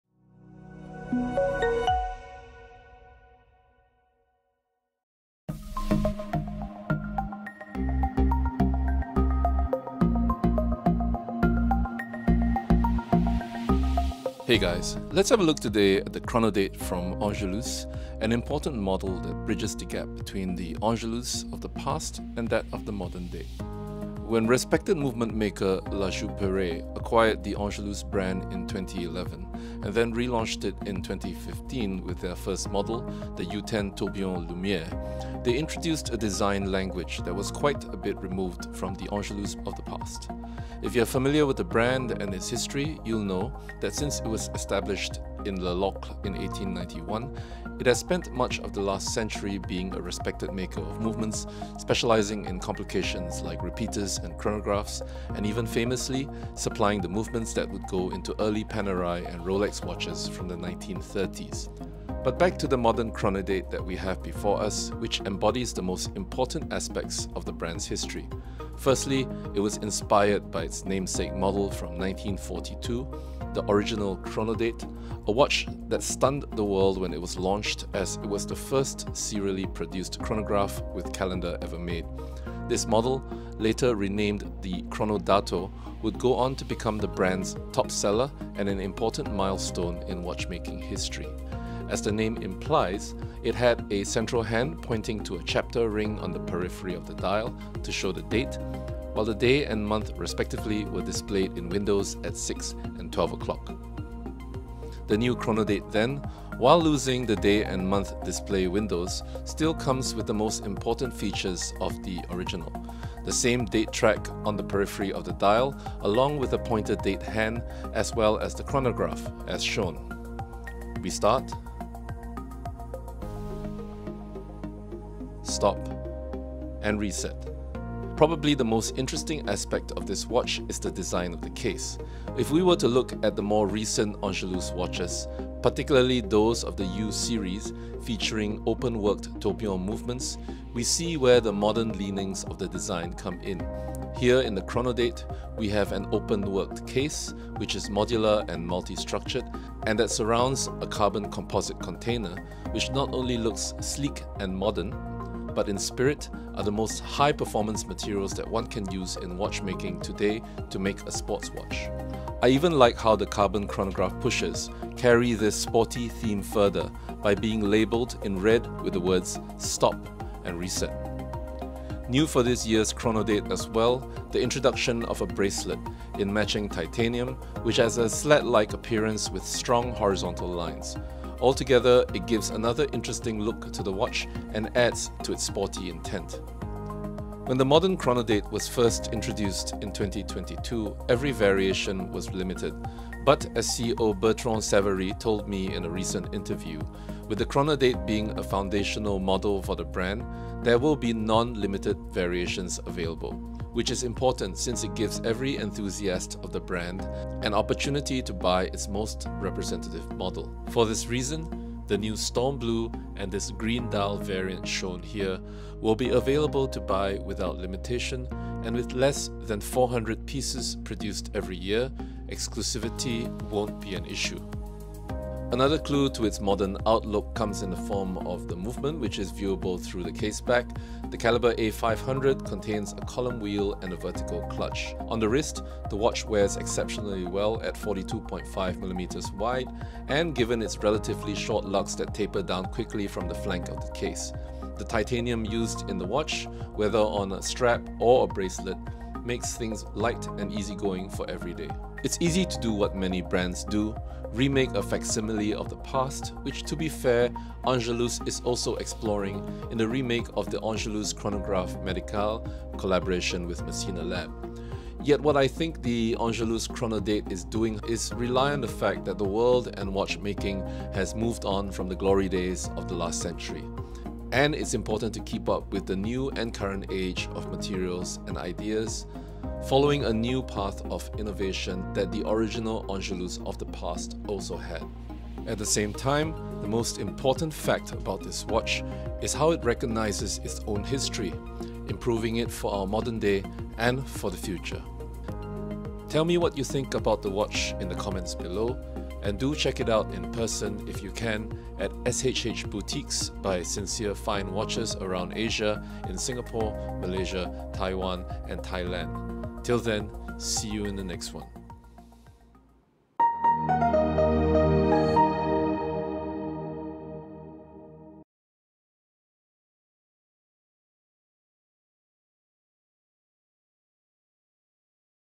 Brothers in Time - The Angelus Chronodate revived after 70 years
The Angelus Chronodate from 1942 was a milestone in watchmaking, being the first serially produced calendar wristwatch with chronograph at the time of its launch.

In this video, find out if the new Chronodate from 2022 takes up the mantle of its big-brother, with its modern design, materials and movement, and if it has what it takes to be the next foundational icon of the new 'Angelus" brand.